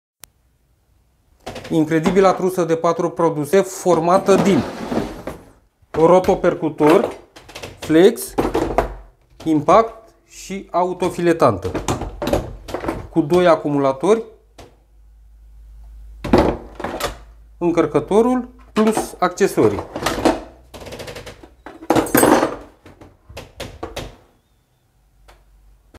Trusa German Meister Rotopercutor, Autofiletanta, Polizor unghiular, Cheie impact, 2 acumulatori 6Ah
Dacă căutați o trusă completă de scule electrice pentru proiectele dvs. de casă sau pentru utilizare profesională, Trusa German Meister este soluția ideală. Cu patru scule puternice și versatil această trusă acoperă o gamă largă de aplicații de găurire, șlefuire și strângere.

Setul Contine 4 Scule German Meister: Cheie impact pe acumulator + Rotopercutor + Polizor unghiular + Autofiletanta + 2buc. baterii 128V 6Ah + încărcător + valiza pentru transport.

Pachetul contine:
1buc Rotopercutor
1buc Autofiletanta
1buc Polizor unghiular
1buc Cheie impact
2buc Acumulatori XGT 128 Vmax 6Ah
1buc Incarcator
1buc Valiza pentru transport

+ accesorii: Protectie si cheie Polizor, maner lateral, bretea fixare